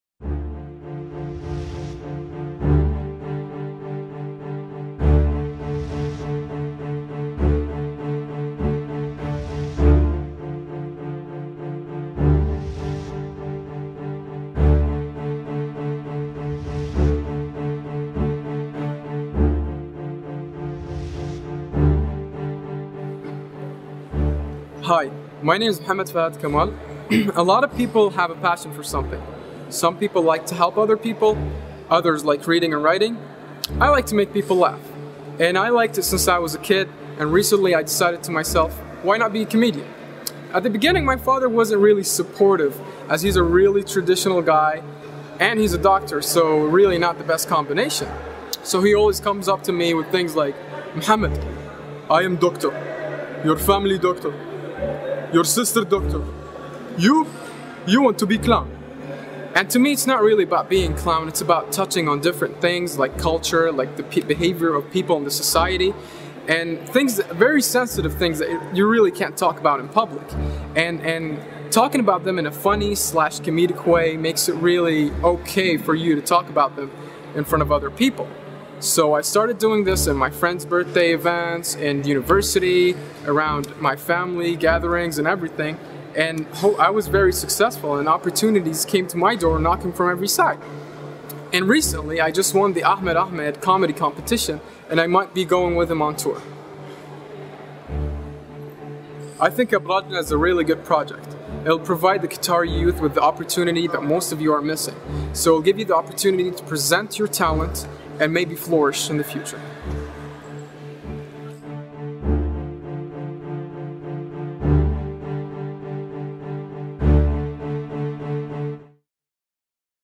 [ الفنان الكوميدي محمد كمال - Standup Comedian Mohammed Kamal ]
مقابله مع الفنان القطري الواعد
محمد فهد كمال
ضمن حمله شخصيات تدعم مشروع أبراجنا

An interview with the stand-up comedian Mohammed Fahad Kamal in "Figures that supports Abrajnna" campaign